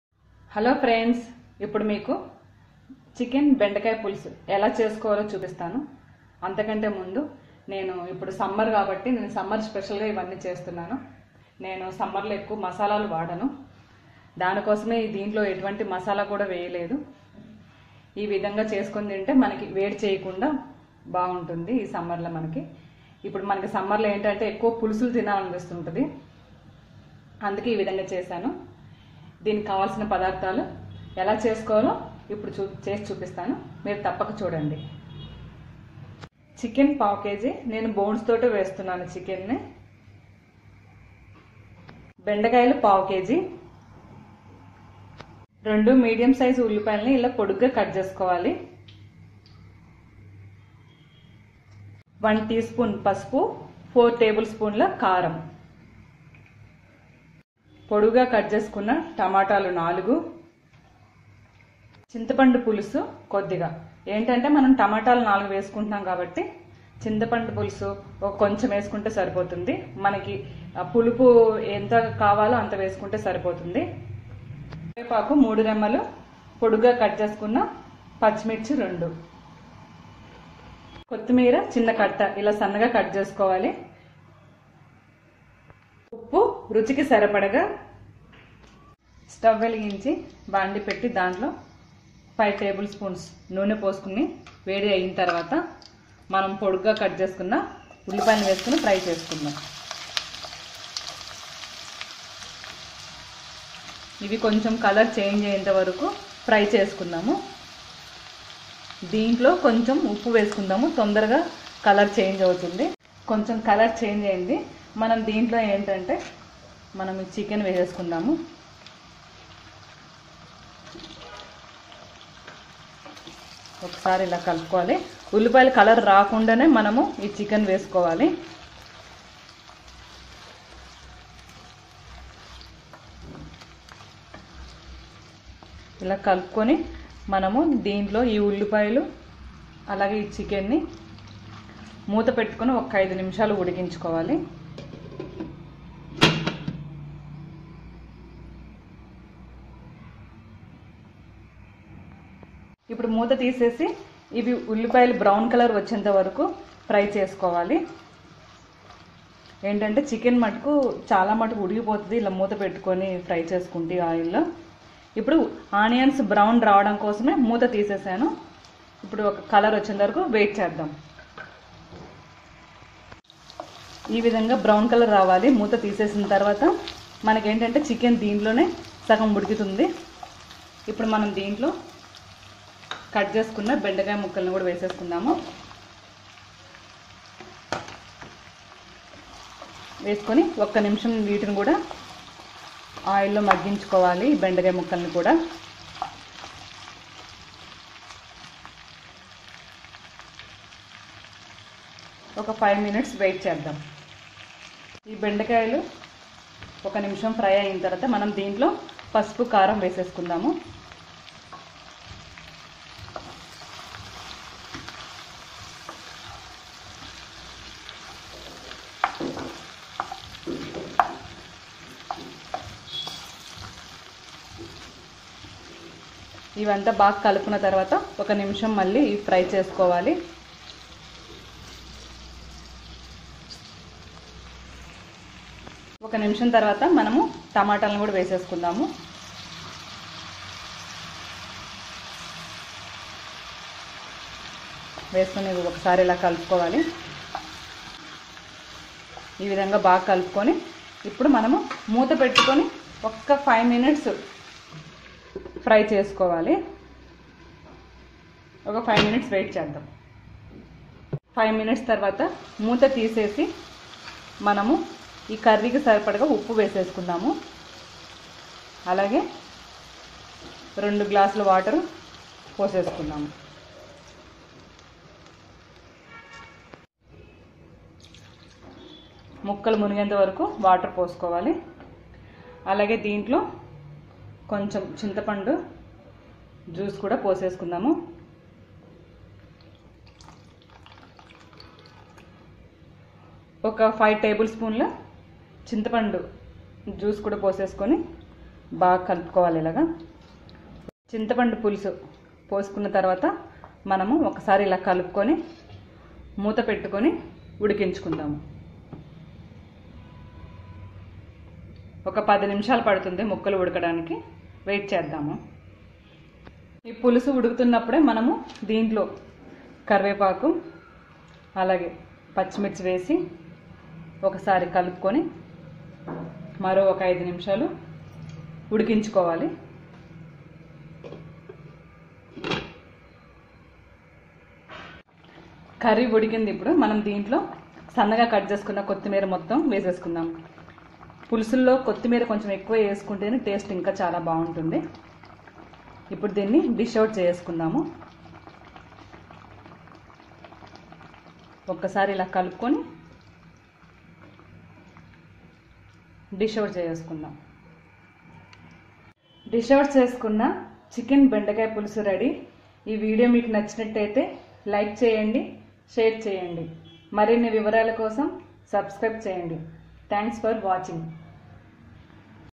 //చికెన్ బెండకాయ పులుసు//రైస్ లోకి ఈ విదంగా చేస్తే చాలా బాగుంటుంది
chicken bendakaya pulusu
chicken bendi pulusu
chicken vokra curry
bendi chicken pulusu
c hicken pulusu

Ingredients

Chicken 1/4kg
Bendakaya 1/4kg
Oil 5table spoons
Onions 2
Tomatos4
Chilli 2
Curry leaves
Salt to taste
1 tea spoon termaric
4table spoons mirchi powder
Coriader mayanksmomsallinone#